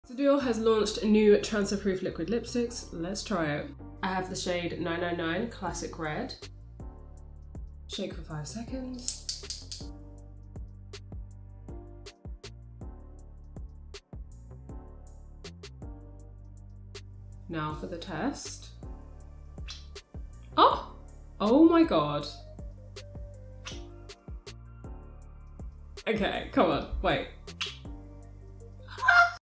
Transfer proof lipstick? Swatching the Dior Forever Rouge
Testing out the new Dior Beauty Forever Rouge Lipstick in the shade 999 Classic Red (gifted pr product)